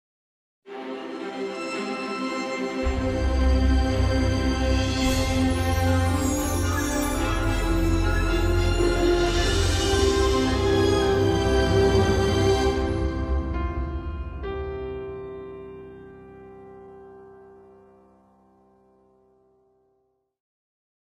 [Fanon] El TV Kadsre Films Logo Music (December 25, 2020-Present)
in christmas day 2020, El TV Kadsre Films got a Update Logo and Music to replaced her previous "glass" logo.

The logo was debuted on 2 animated movies including Disney/Pixar's Soul & Universal/Dreamworks's The Croods: A New Age (Both are which was Distributed by El TV Kadsre Films in El Kadsre)

For Jedidiah Cudby, Blue91233 and Arthur Persson (Alp Entertainment)